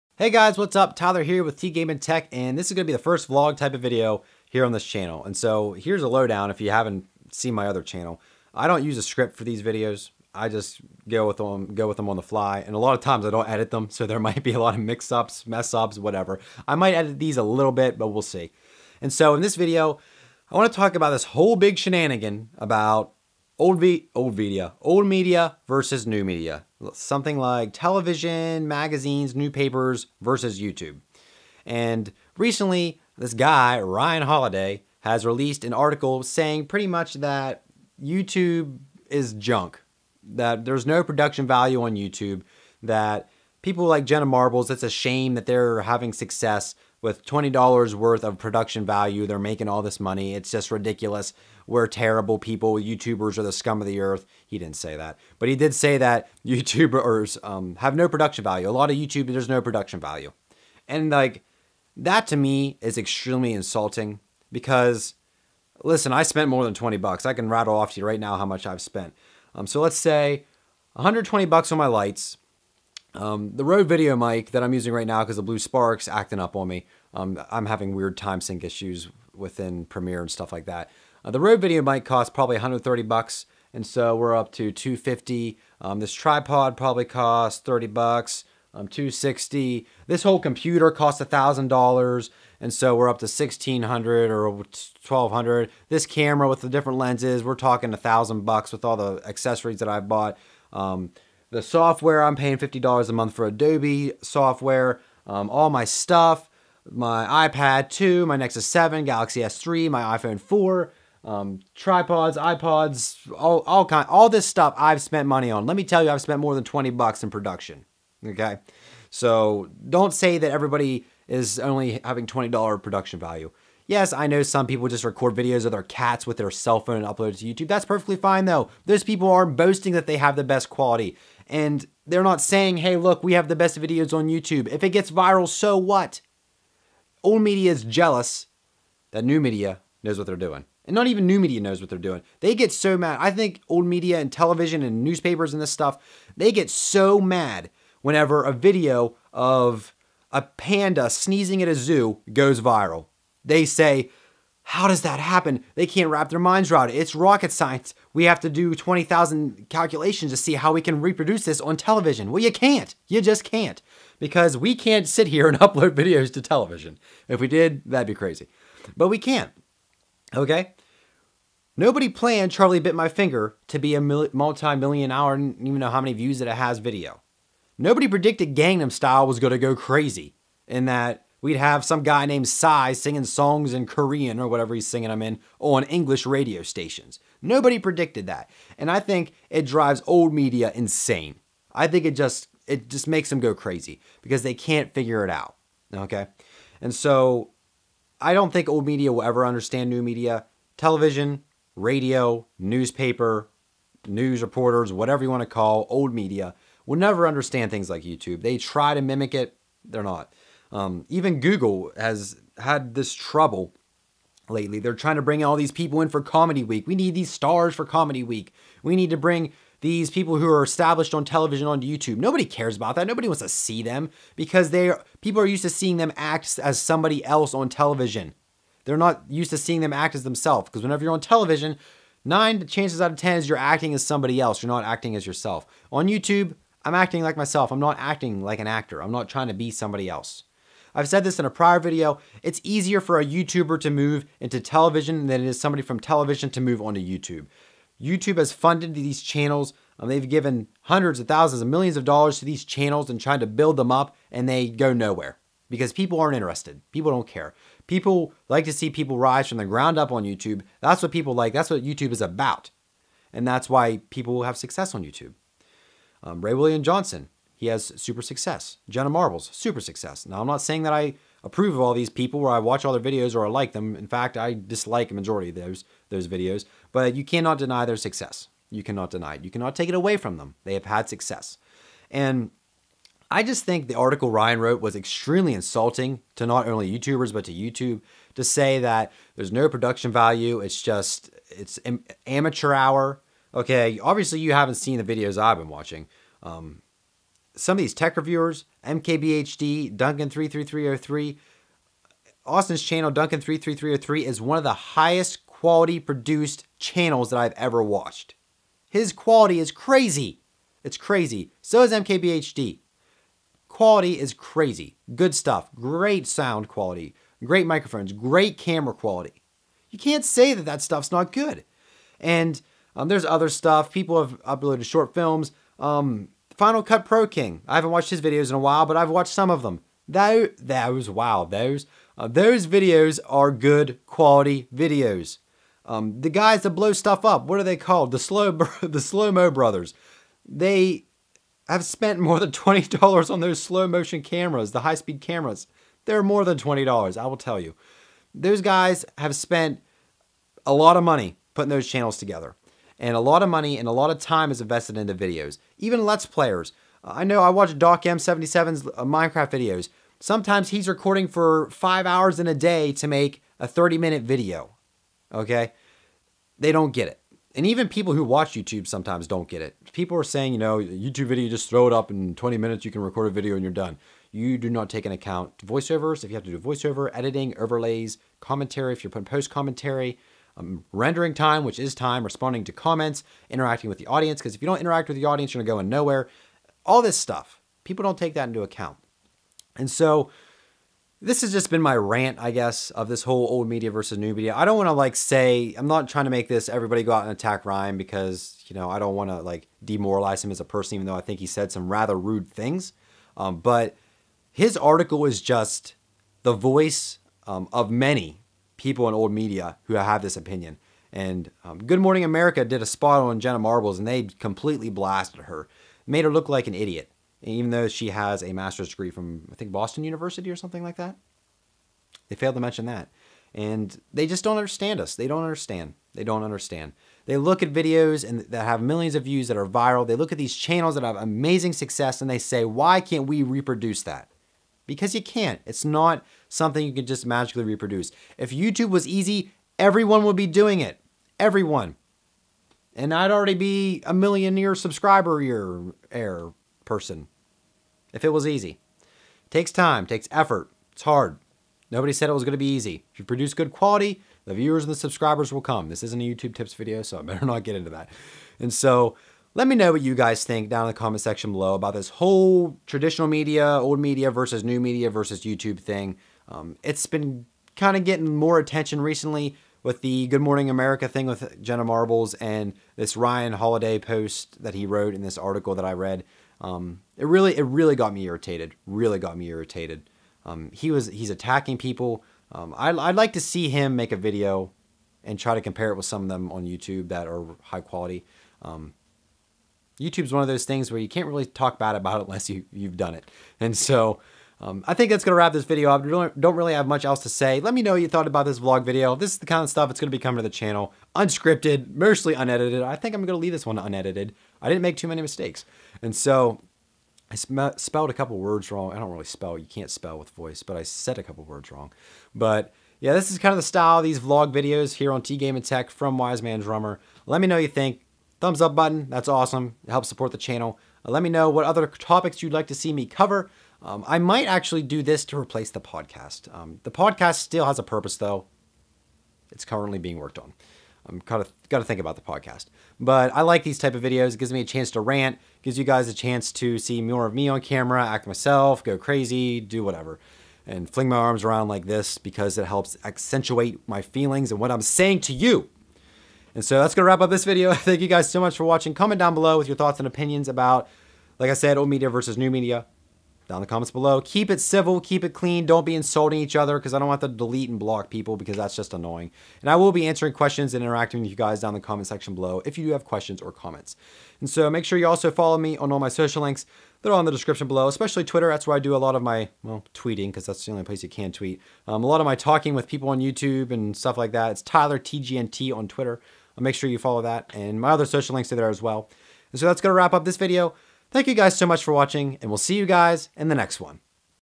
Old Media vs New Media
Rant mode: ACTIVATED.

I talk about this whole old media vs new media buzz that is going around the internet and reference Ryan Holiday's article (link below) where he asks "Why are YouTube videos so terrible" and uses words like "amatuer" to describe videos. I also talk about Good Morning America and how they blasted Jenna Marbles.

Overall, old media is jealous of new media and they want to be new media but can't figure out how...and it's because there is no "secret recipe" to the success people are having on new media.